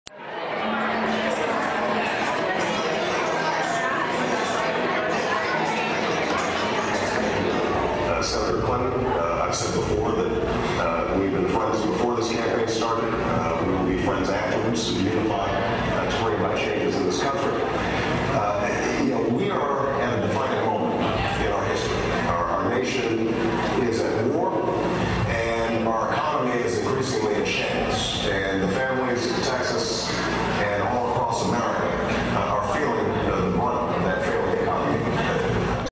The scene the LBJ School watch party, 02.21.08
Students, faculty and alumni of the LBJ Public Affairs School gathered in the basement of the Conference Center for a debate watching party after the Barbara Jordan National Policy Forum, held earlier in the day. More than 50 people attended the gat